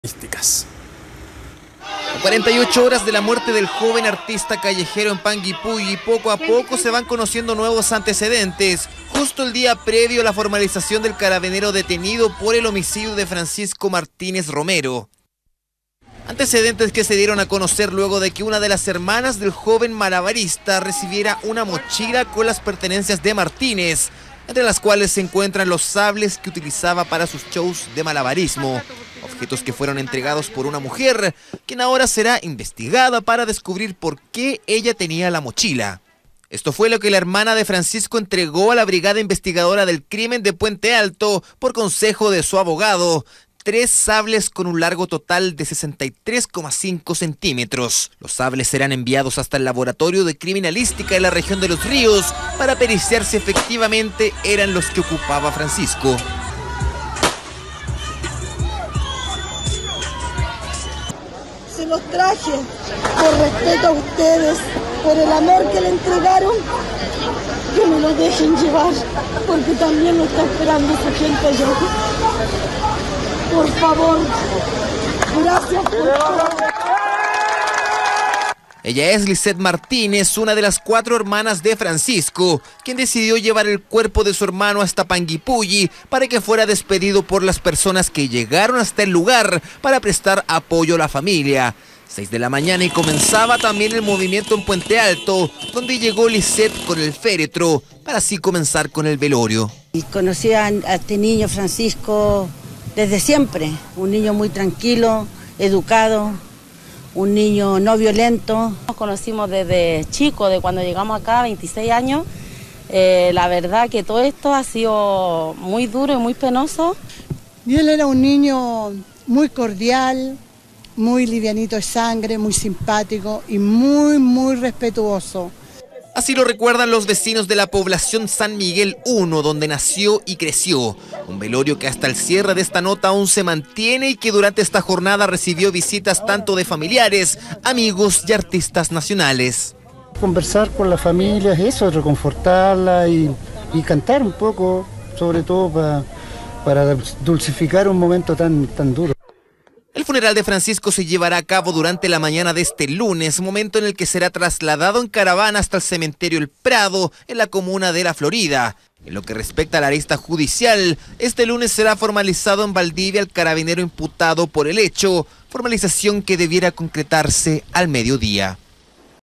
PDI pericia sables de malabarista asesinado por un funcionario de Carabineros en Panguipulli
Este domingo se realizó el velorio del malabarista Francisco Martinez, en Puente Alto. Además, la PDI confirmó que la hermana de Francisco entregó los sables que supuestamente él utilizaba para sus presentaciones. Los elementos serán analizados por el laboratorio de criminalística de la institución para esclarecer su origen y características.